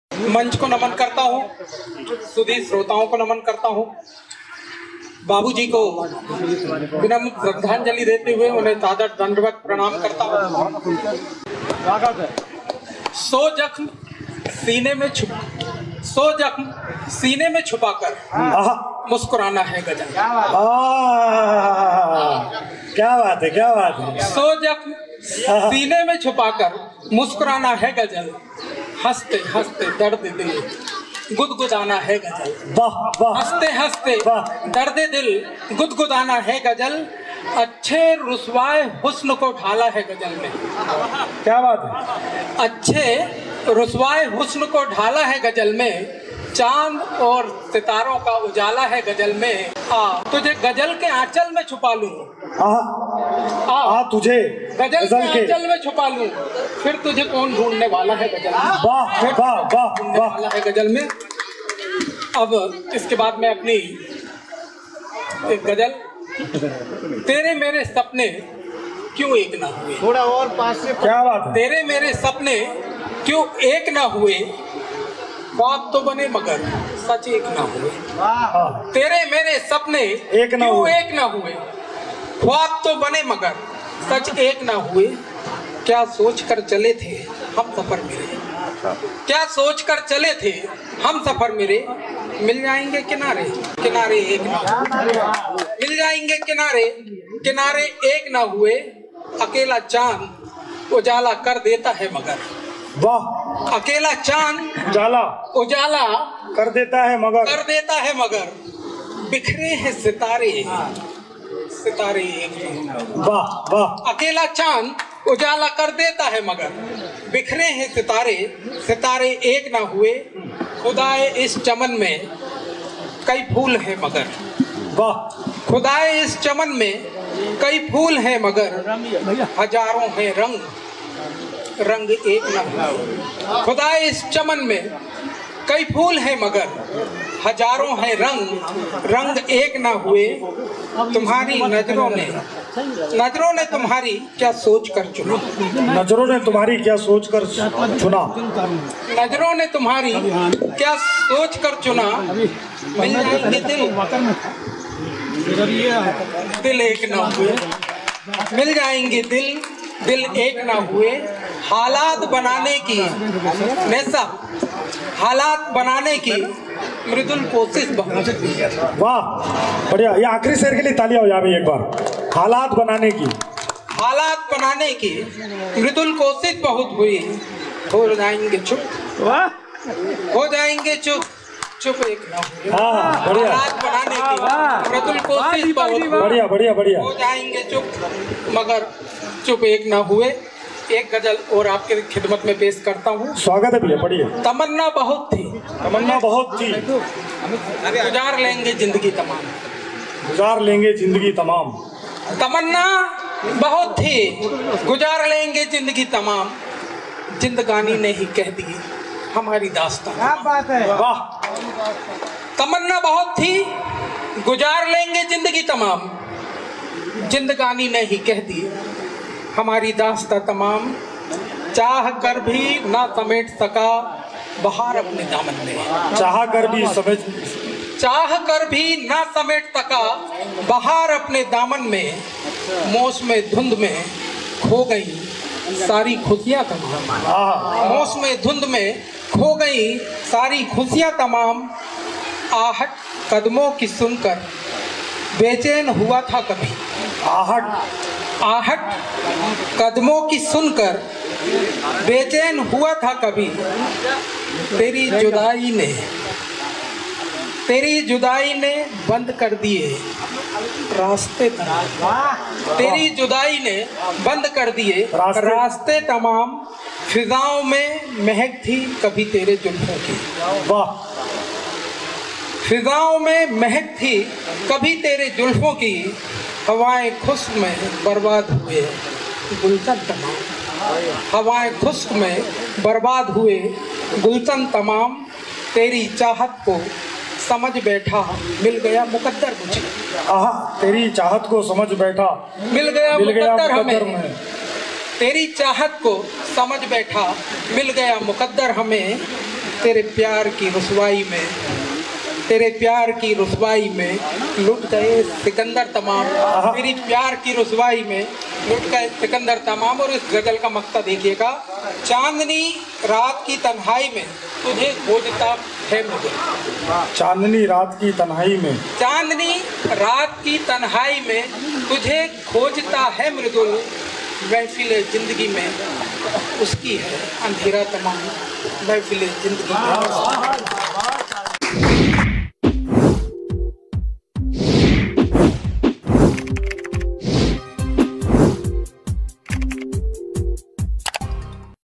मृदुल अमरपुरी । बेहतरीन शायर के बेहतरीन अशआर । खूब बजीं तालियाँ । Mridul Amarpuri
निशान काव्य महोत्सव
एवं
अखिल भारतीय विराट कवि सम्मेलन
नवरात्रि के पावन पर्व के उपलक्ष्य में कवियों का महाकुंभ
शनिवार, दि. 2 अप्रैल 2022
स्थल : मेला ग्राउंड, निशान जानोजी, तहसील चाँद, जिला छिंदवाड़ा (मध्यप्रदेश)

प्रथम सत्र : काव्य महोत्सव
--ः ग्रुप अ :--

द्वितीय सत्र : अखिल भारतीय कवि सम्मेलन

आयोजन समिति
o डॉ. अर्पण जैन ‘अविचल’, राष्ट्रीय अध्यक्ष, मातृभाषा उन्नयन संस्थान
सूत्रधार/आयोजक
o श्री हरिप्रसाद नागवंशी